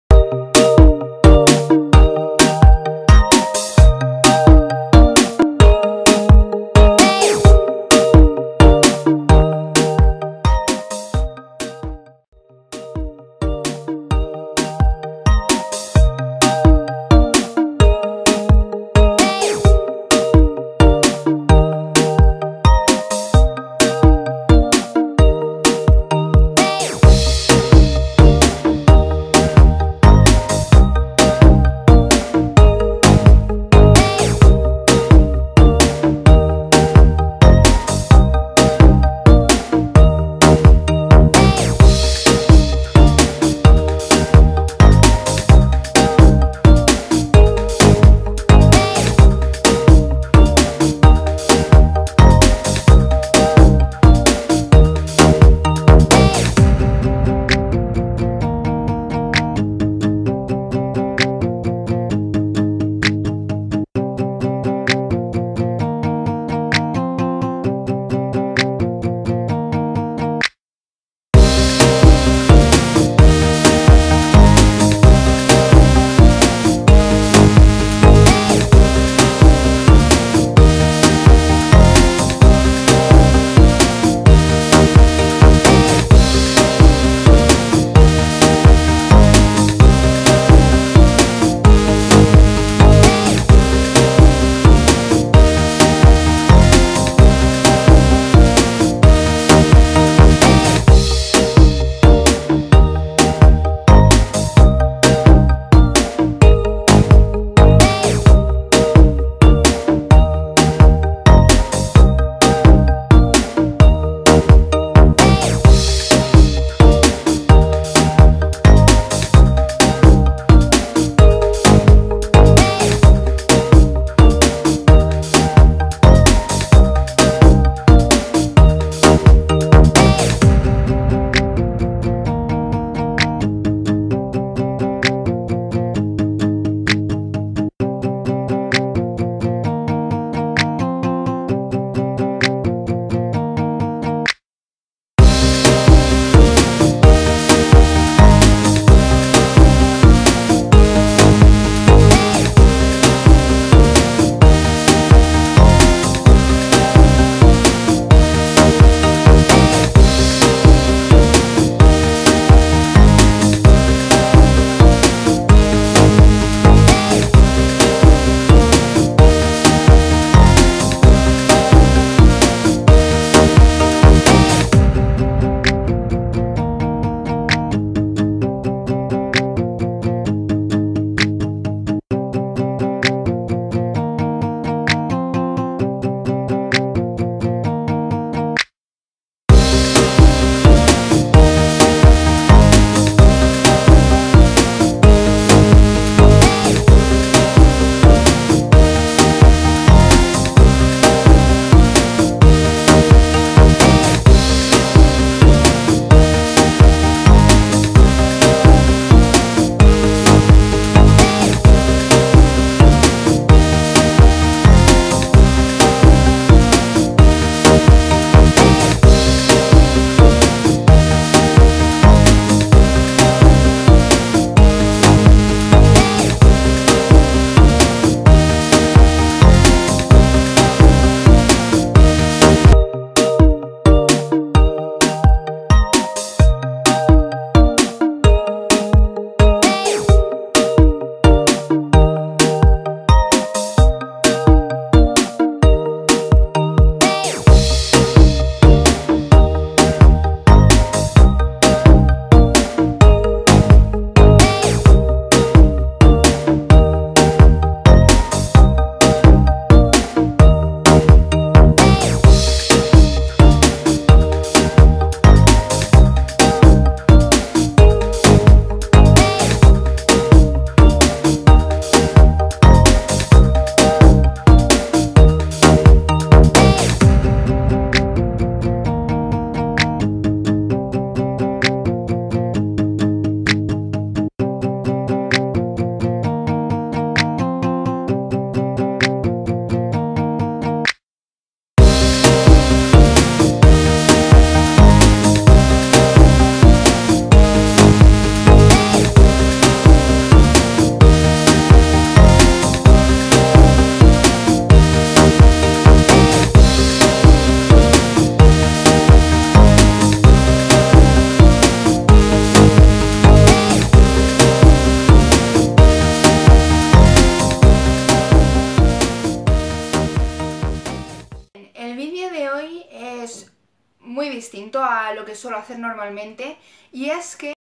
MAQUILLAJE Ahumado marrón | Makeup tutorial
Hola a todas!!! En el vídeo de hoy os enseño un ahumado marrón suuuuper bonito y elegante, y aunque no tiene voz, los productos los tenéis en pantalla :) Un beso preciosas!!